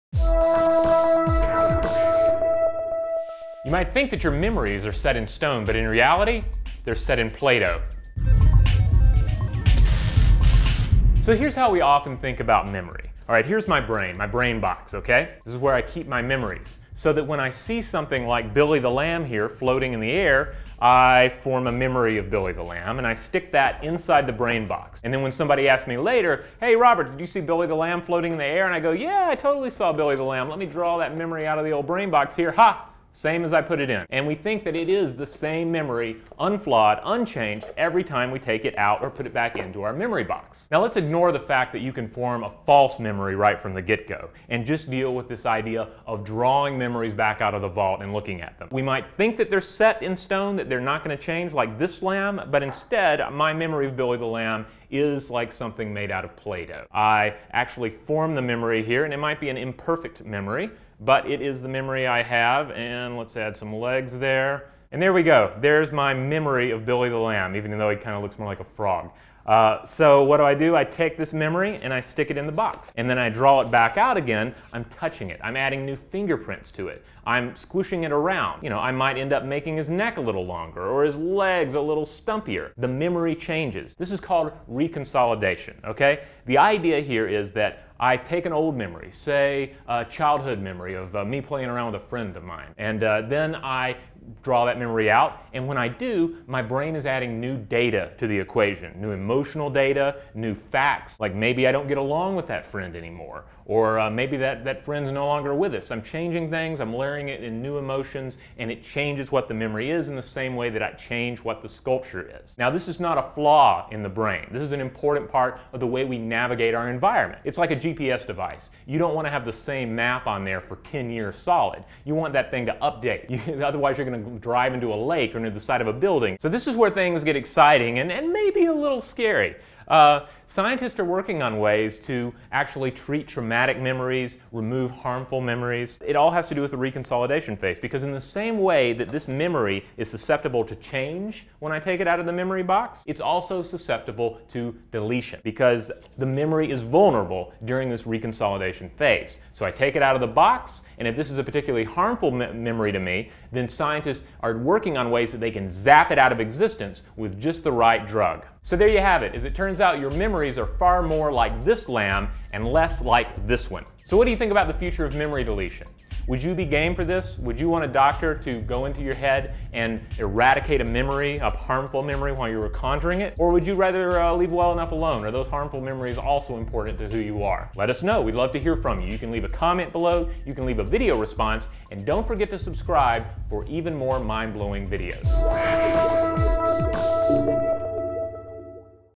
How Malleable is Our Memory? - Epic Science #26
As it turns out, your memory isn't set in stone. It's a Play-Doh sculpture that changes a little every time you retrieve it from the old brain box. Learn more about doughy memories in this video from Stuff to Blow Your Mind.